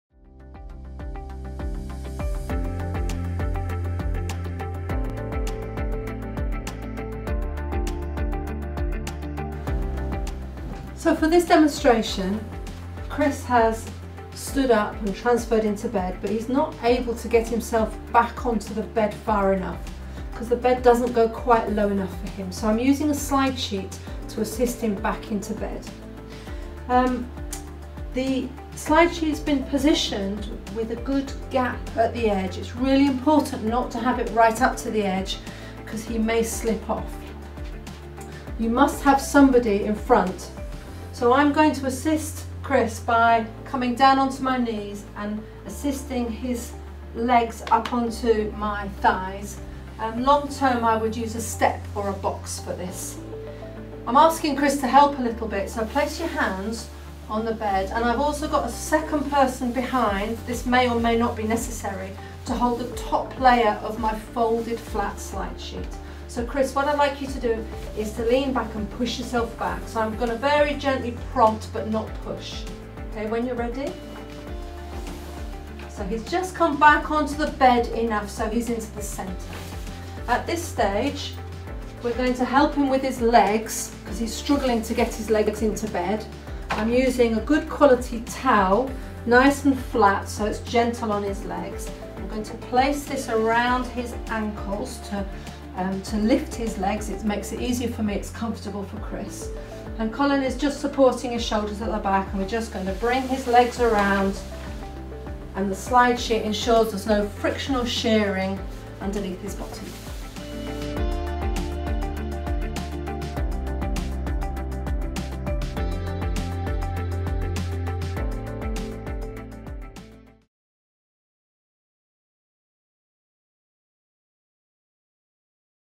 Supporting a Patient Into Bed Using a Folded Flat Slide Sheet
Moving & Handling Practitioner Julia Love demonstrates supporting a patient into bed using a folded flat slide sheet. Features a Cromptons Healthcare by MIP Slide Sheet.

Explore our Slide Sheet Learning Hub to find more education and training support tools

You can find out more about our products on our